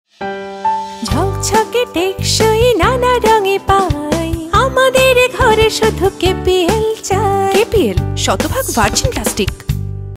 KPL Deluxe Rack TV Commercial
KPL Unique Rack is manufactured using 100% Virgin Plastic raw materials which ensure product strength, durability and long lasting color.